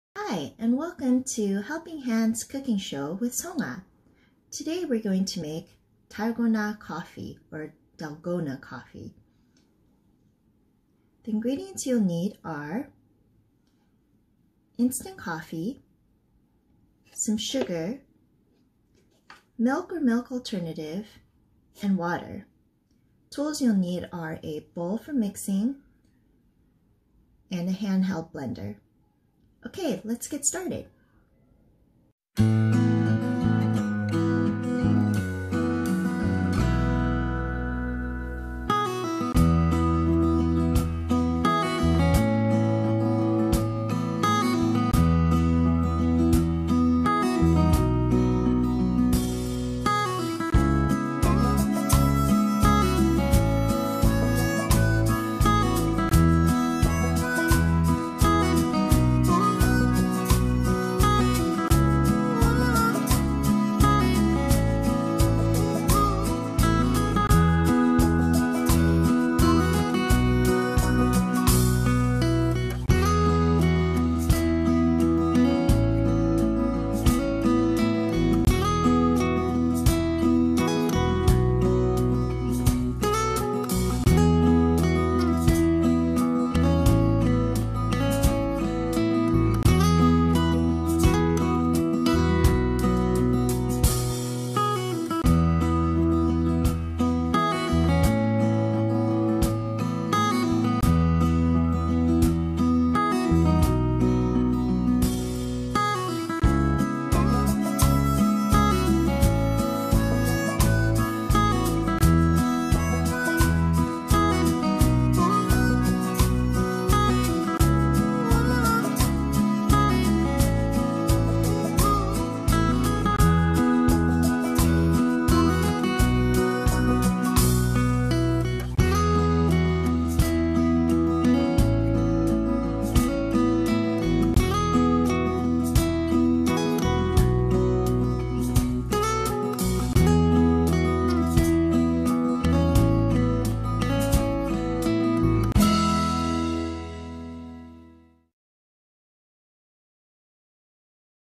Dalgona Coffee
Helping Hands Cooking Show Episode 2: How to make Dalgona coffee